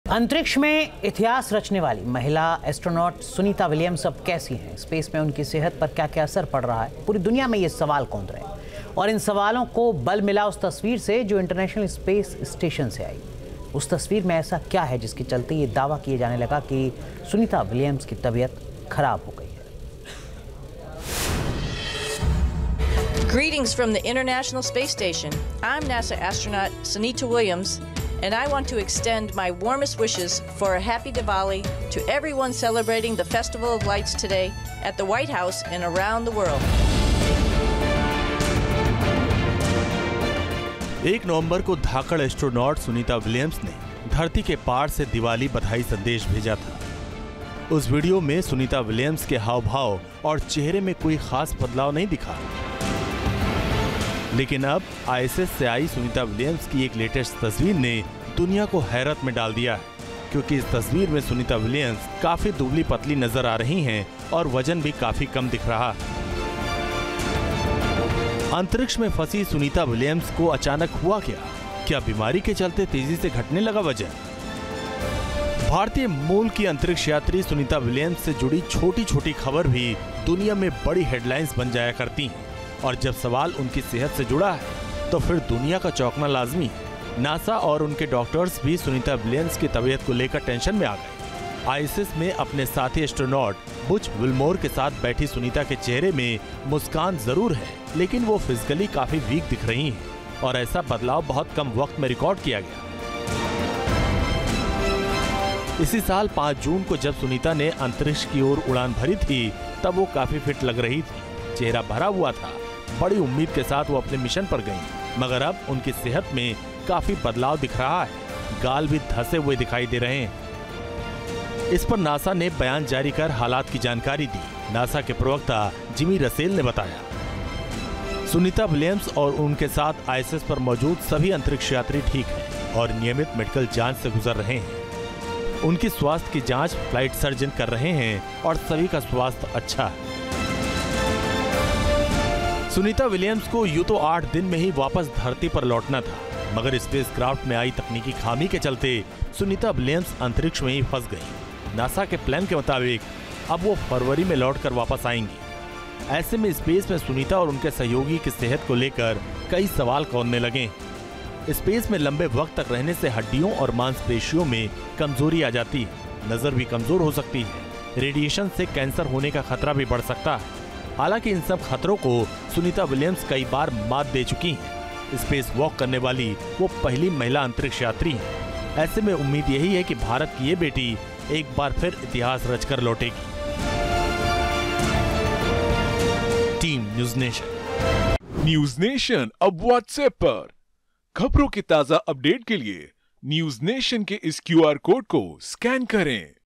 Sunita Williams Update: अंतरिक्ष में फंसी सुनीता विलियम्स, तस्वीरें देखकर चिंता में हेल्थ एक्सपर्ट
News State Apps डाउनलोड करें:
Download Android and iOS Apps for more updates: